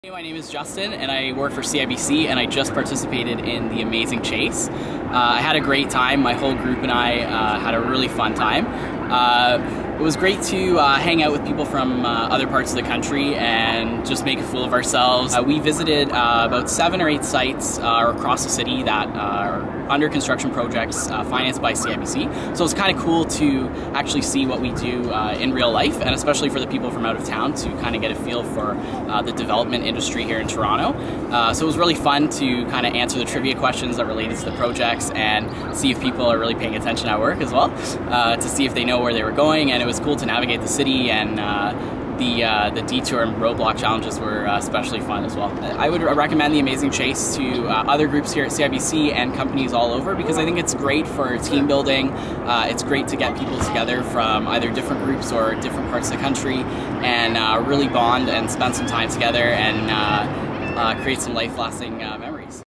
Amazing Chase - Video Testimonial - Outback Team Building & Training
The Amazing Chase is one of the most popular group activities offered by Outback Team Building & Training. This is great team building event or conference activity for groups of all sizes. It can also double as a sightseeing event for groups visiting a city for the first time.